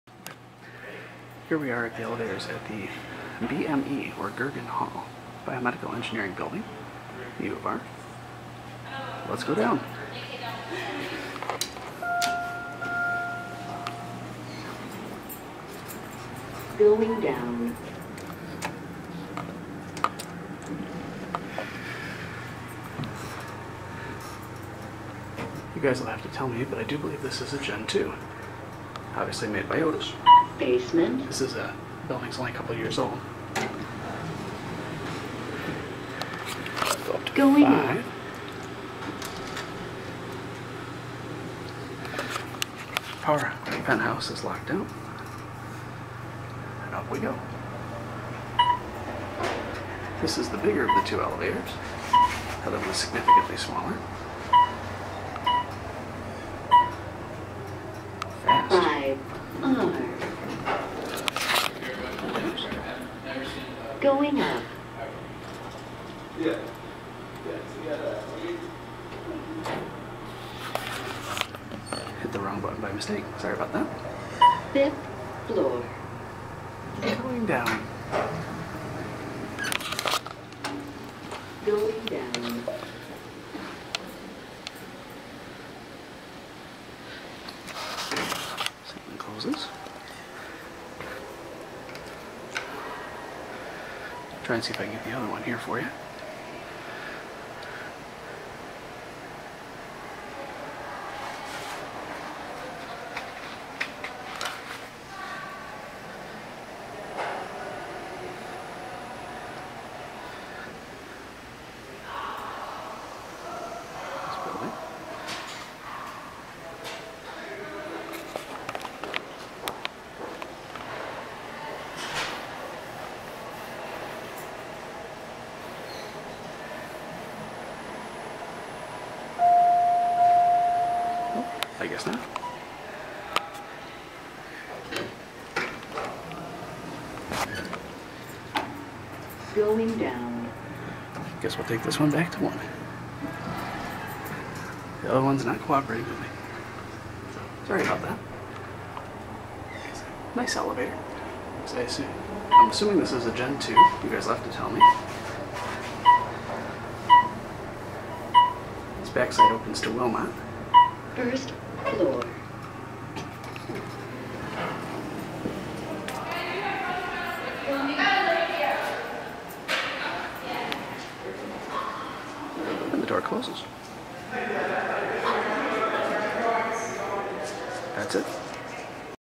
OTIS traction elevator in the BME building
OTIS traction Gen2 elevator in the BioMedical Engineering/Optics building.